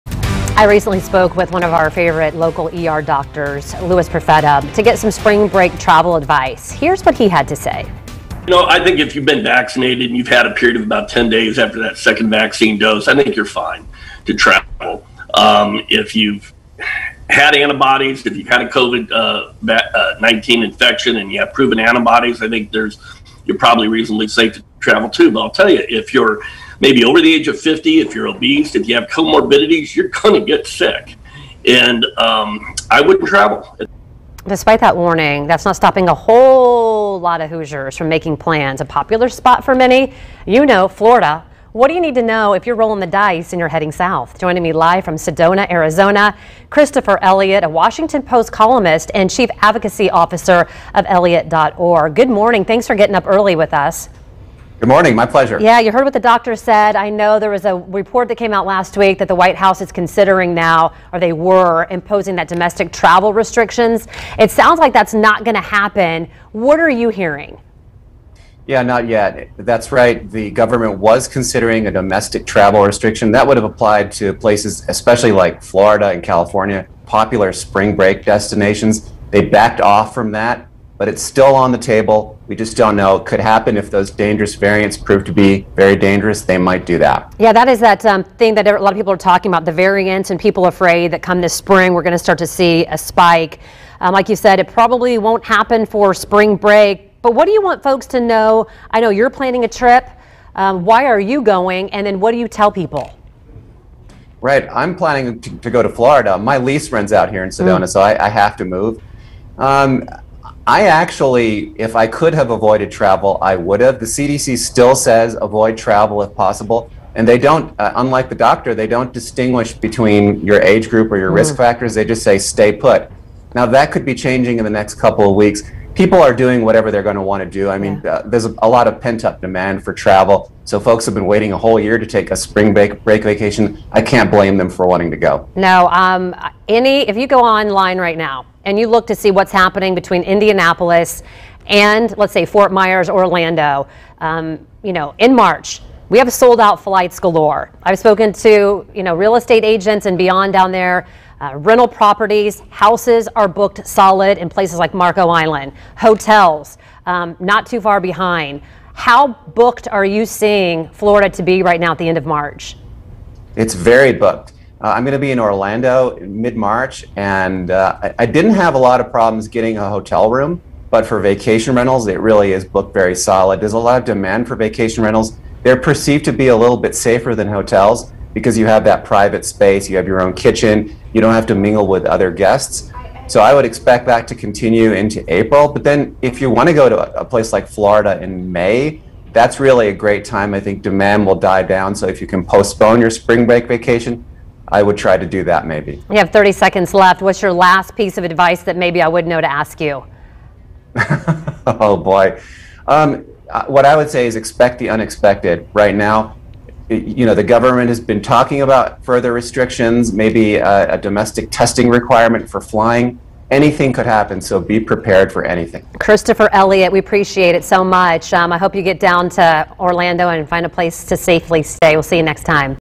Spring Break Travel Advice
Staying safe while vacationing in Florida amid pandemic.